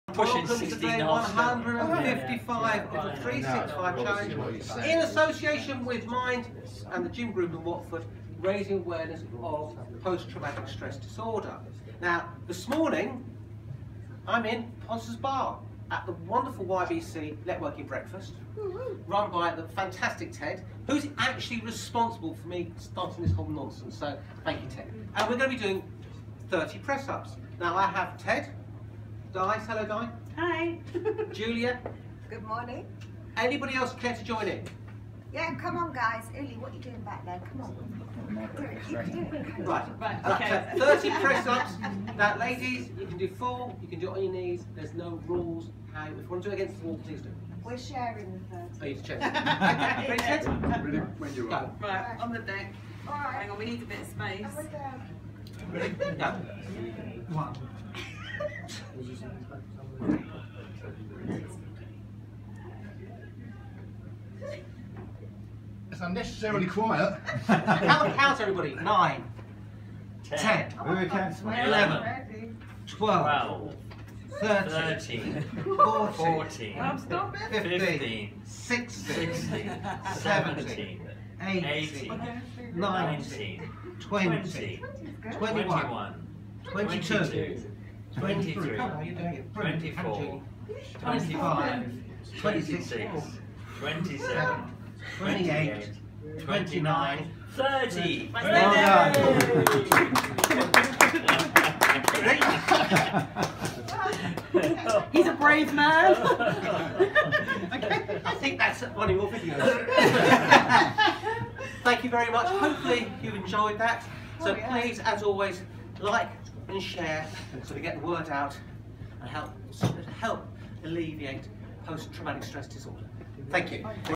Day 155 of The 365 Challenge in association with MIND & The Gym Group Watford
A daily session of The 365 Challenge can be completed anywhere in the Gym, in the park, at home or here at a YBC business network breakfast